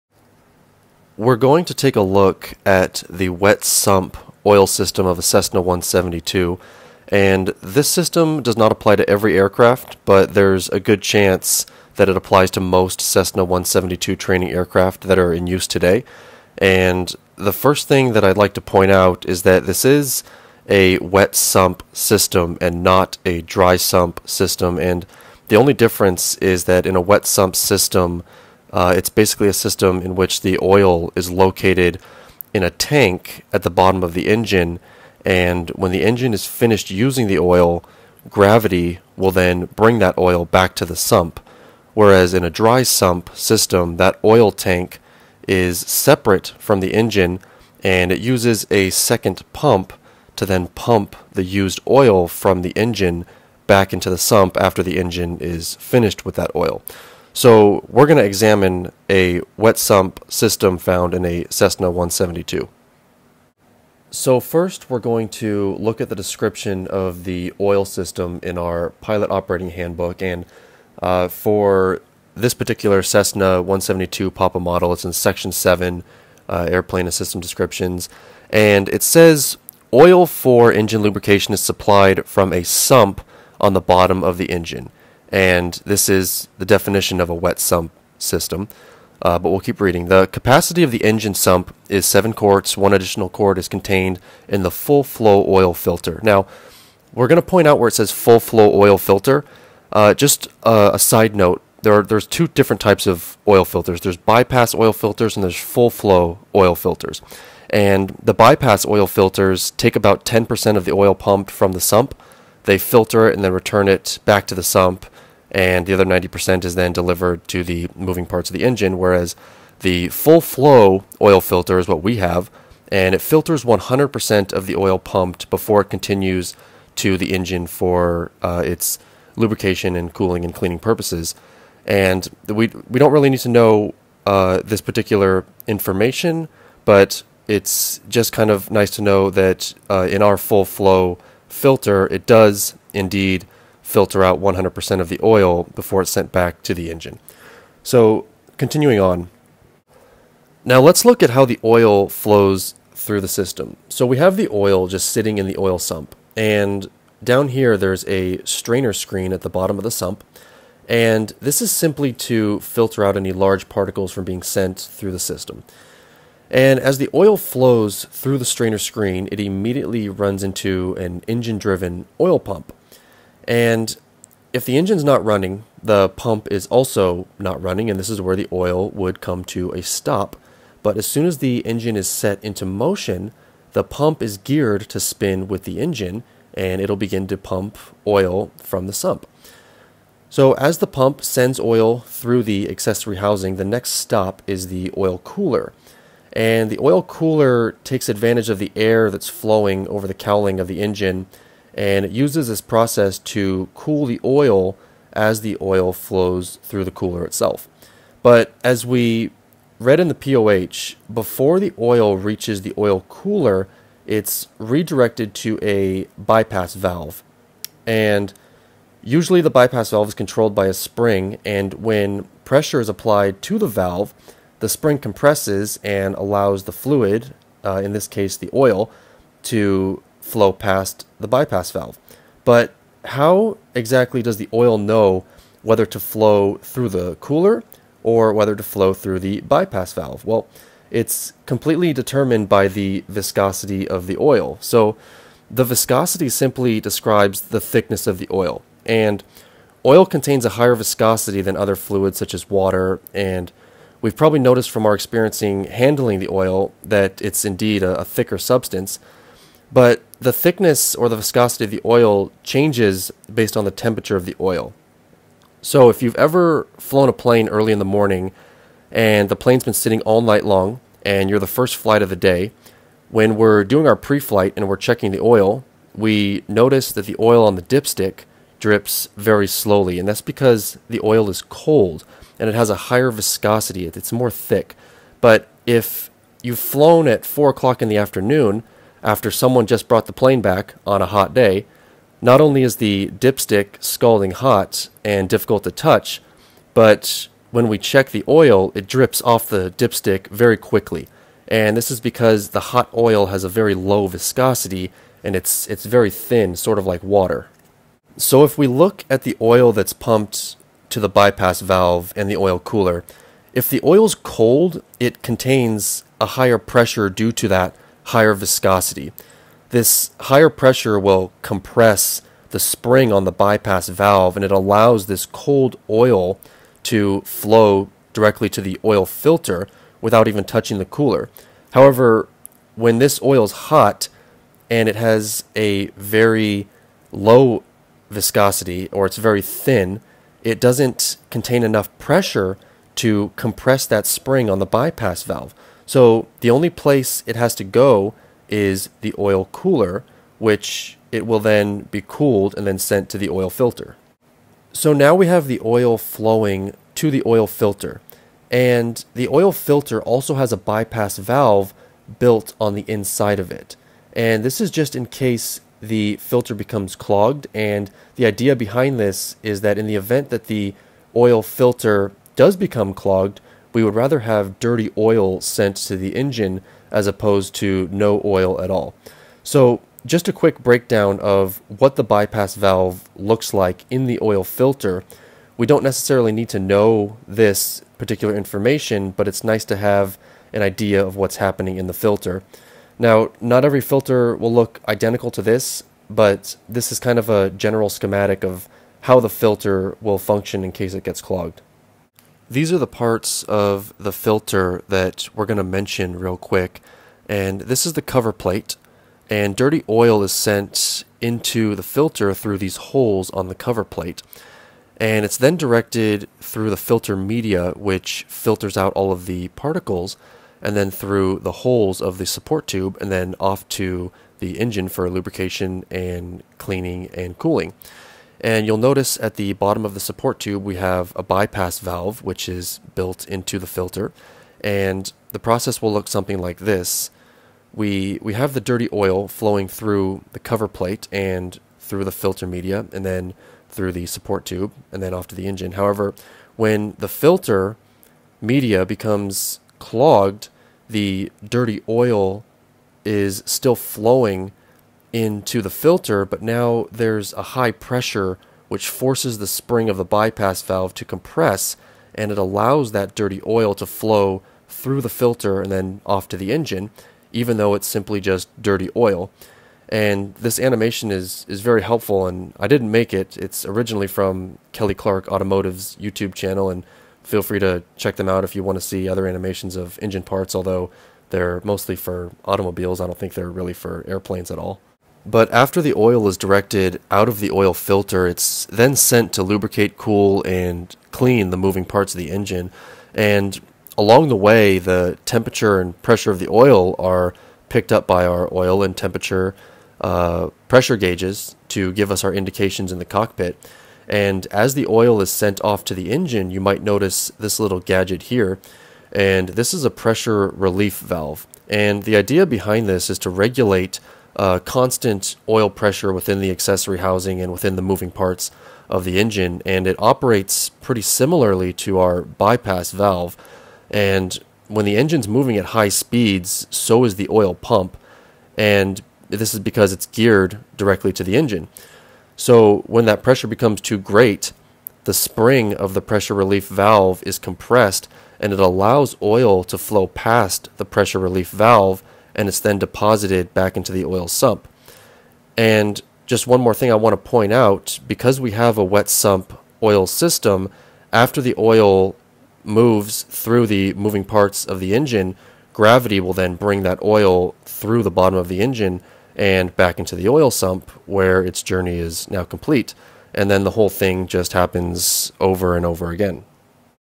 Oil System Cessna 172 | Aircraft systems
Wet sump oil system of Cessna 172P. Checkride prep. Aircraft systems. ￼